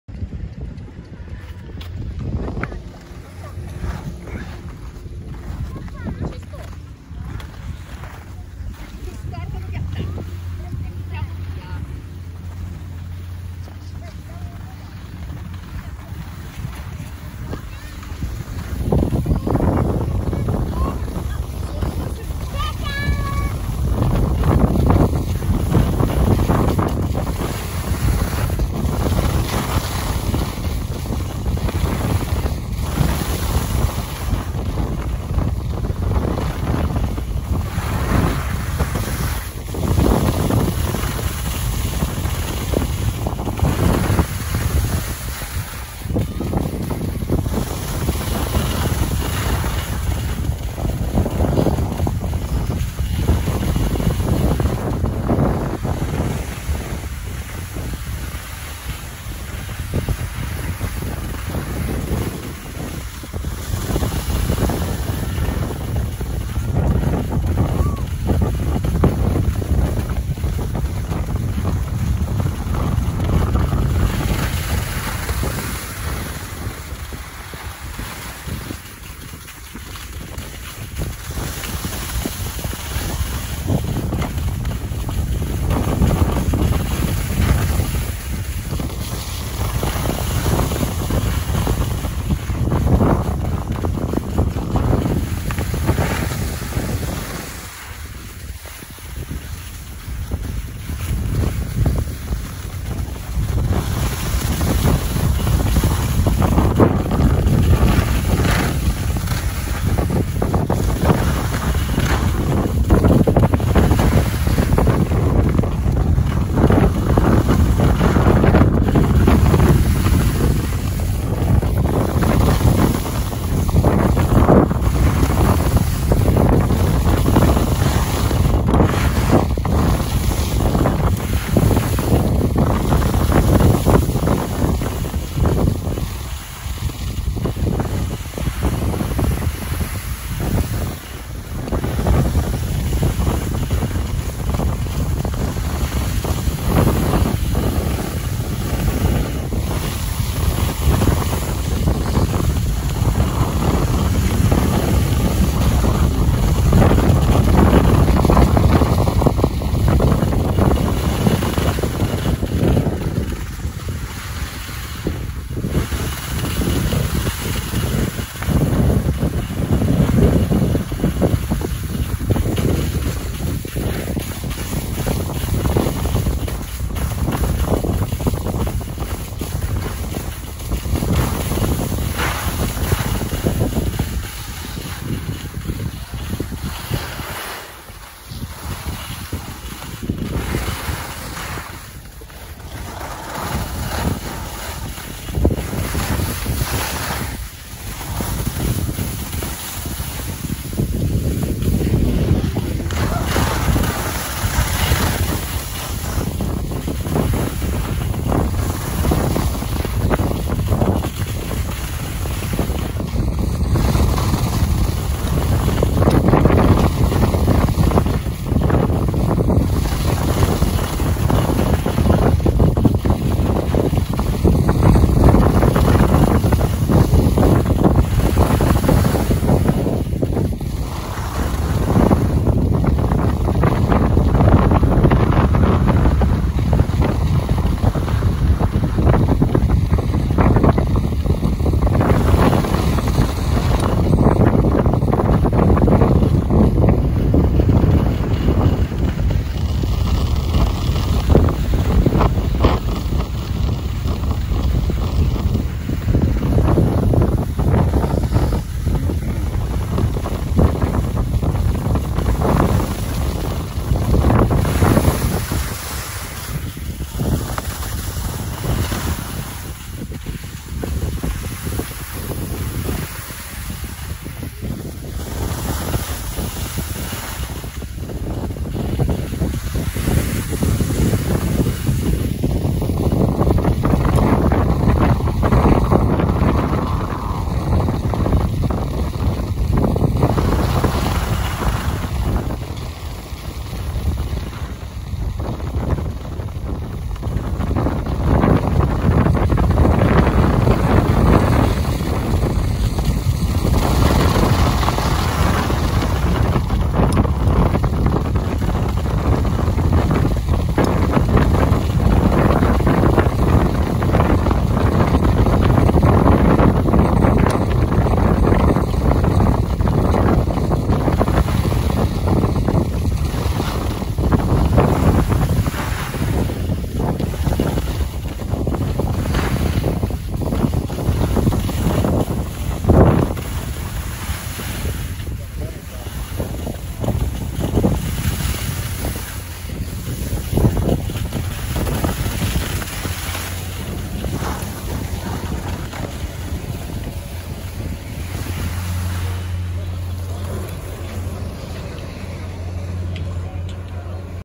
Sciare Passo San Pellegrino: Pista Ciadin, numero 50
Sciare Passo San Pellegrino

Scopriamo ora la Pista Ciadin, numero 50 al Passo San Pellegrino. Tutto su questa skiarea qui